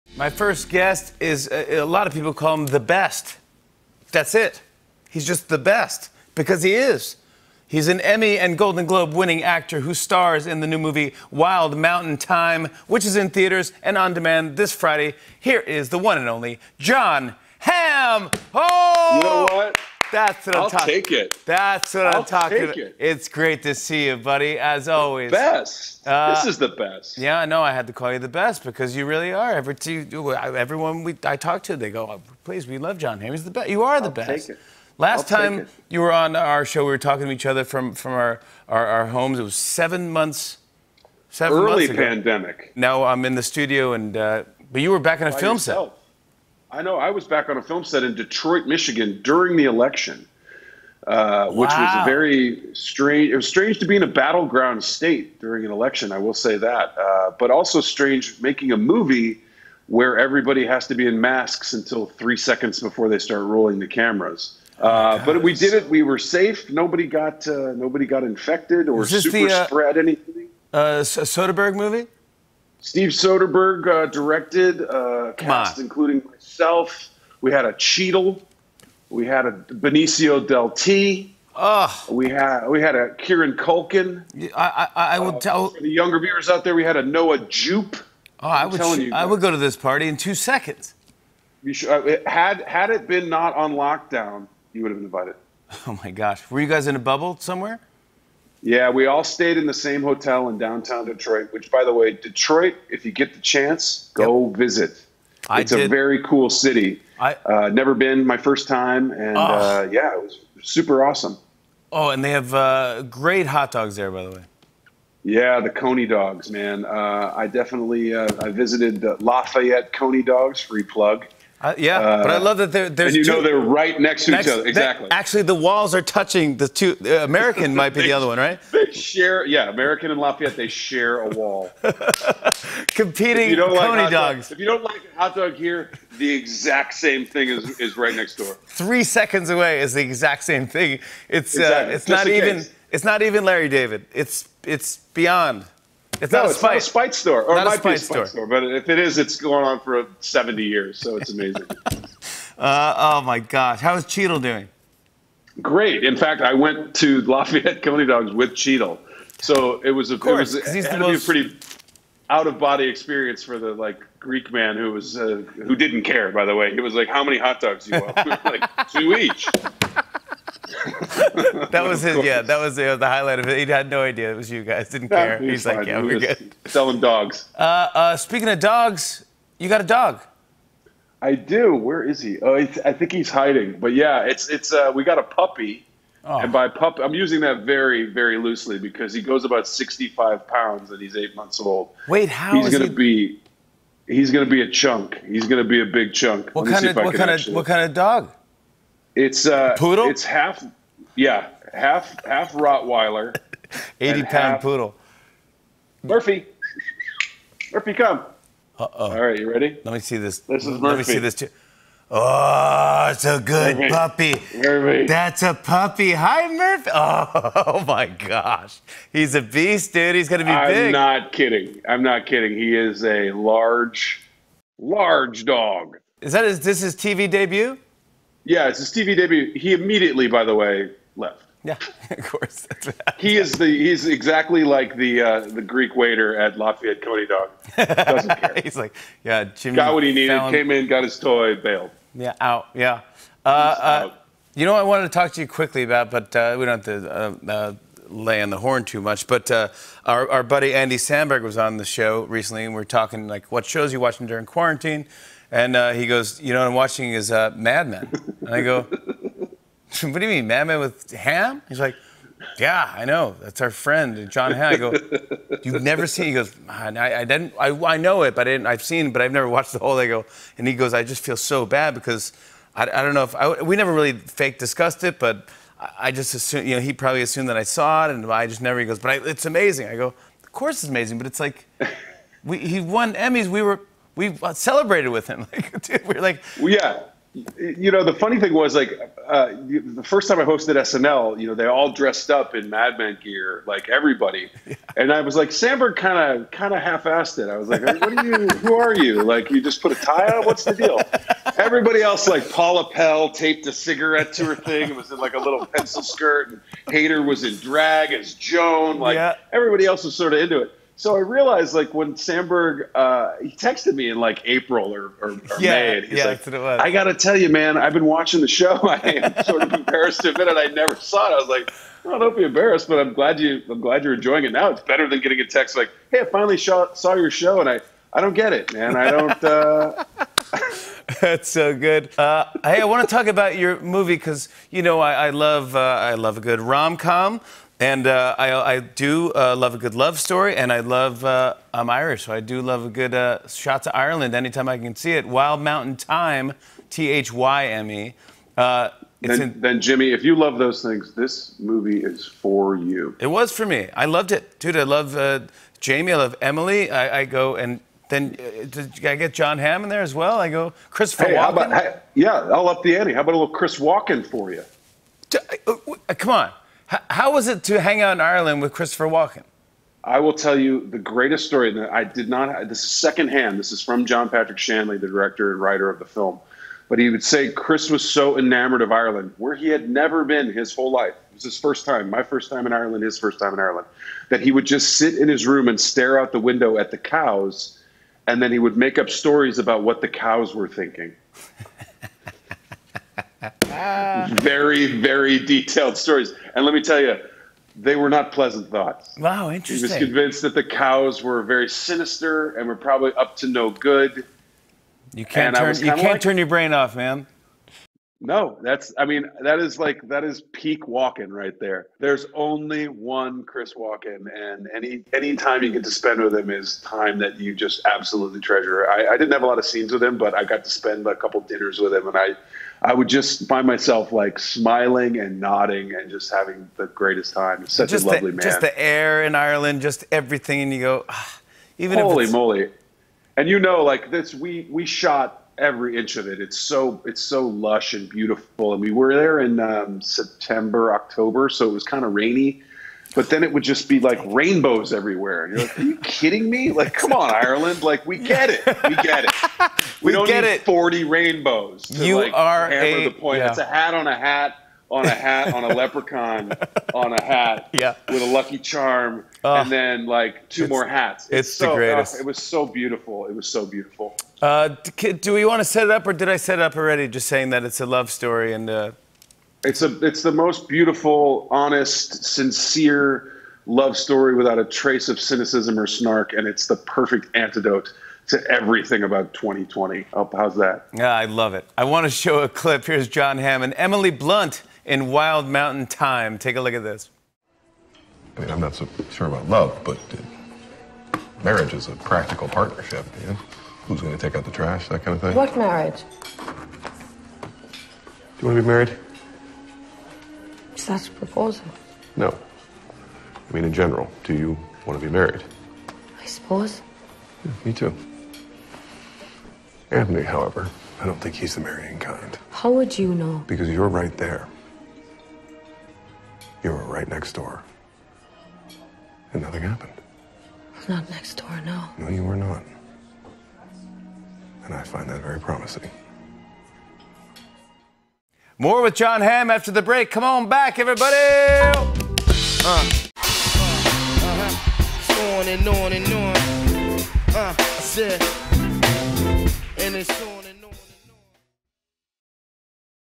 Andy Samberg Apologized to Jon Hamm for Never Watching Mad Men
Jon Hamm talks about exploring Detroit with Don Cheadle, getting a puppy during the pandemic and filming Wild Mountain Thyme in Ireland with Christopher Walken.